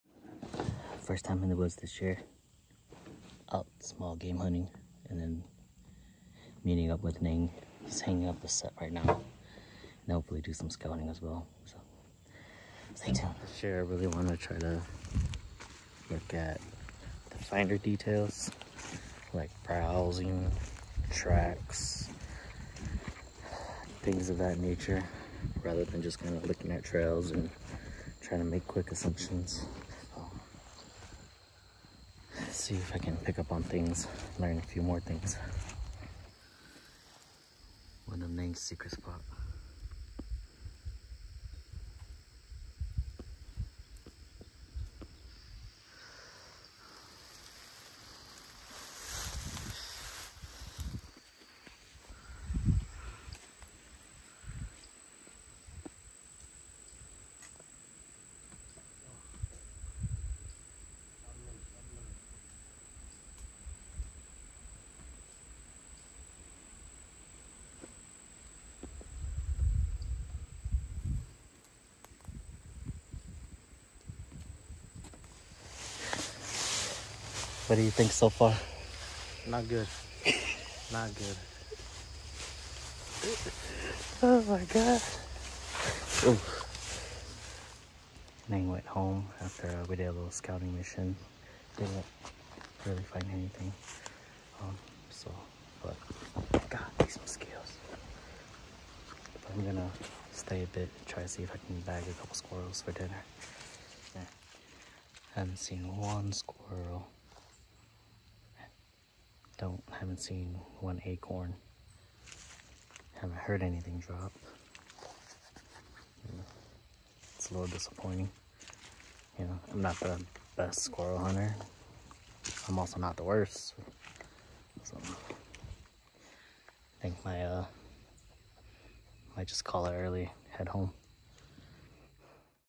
Vlog 23 - First 2024 Scouting Trip
First scouting mission for 2024 and Neng hangs a stand.